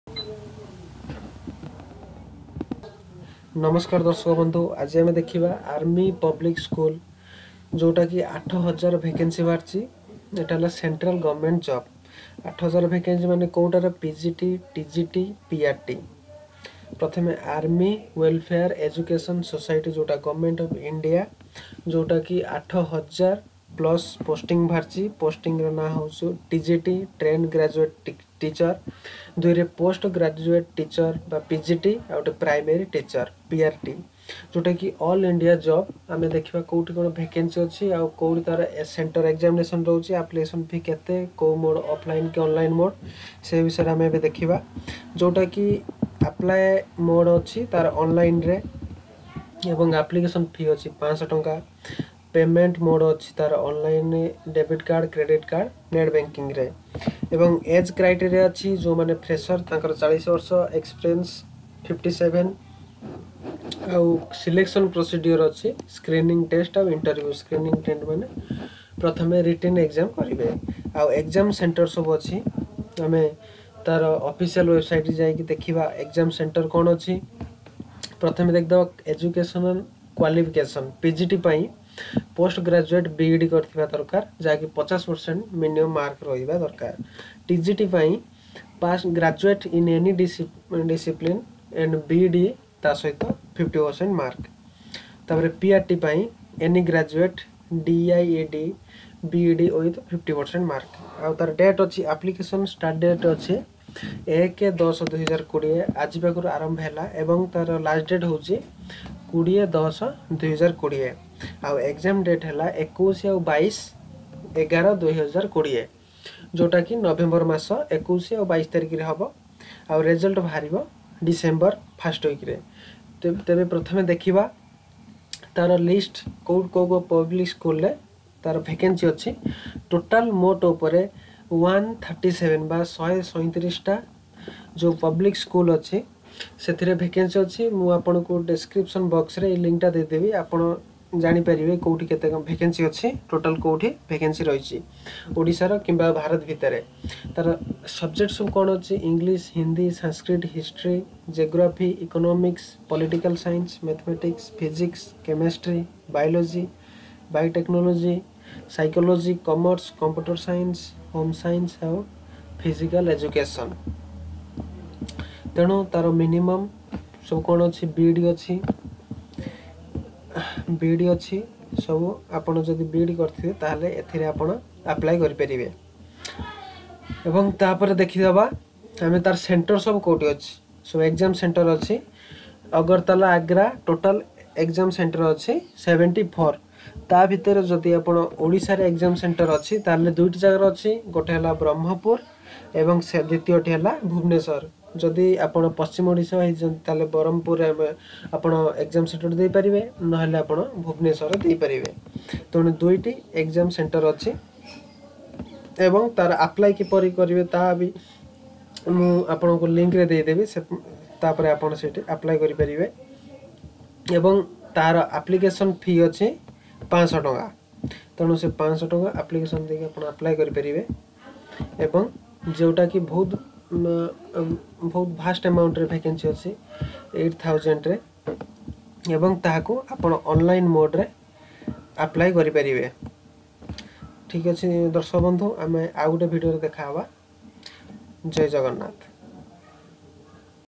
ALL INDIA TEACHER VACANCY 8000+ || FOR BED PASS CANDIDATE APPLY || Army Public School PGT/ TGT/ PRT
AllINDIATEACHERVACANCY 8000+ || ForBEDPass CANDIDATE APPLY || Army Public School PGT/TGT/PRT
Organization Name : Army Welfare Education Society (Govt of India)

Total number of Vacancies : 8000 (Approximately)

Post Name :

TGT (Trained Graduate Teacher)
PGT (Post Graduate Teacher)
PRT (Primary Teachers)
Job Location : All India

Apply Mode : Online

Application Fee : Rs.500/-

Payment Mode : Online (Through Credit Card/ Debit card/ Net Banking)

Age Criteria :

For Fresher Candidates : Below 40 yrs

For Experienced Candidates : Below 57 yrs

(Age relaxation is applicable as per govt rules)

Selection Process : Screening Test/ Interview
Important Dates :
Application Start Date 01/10/2020
Application Last Date 20/10/2020
Exam Date 21 & 22/11/2020
Date of Result 02/12/2020 (Tentative)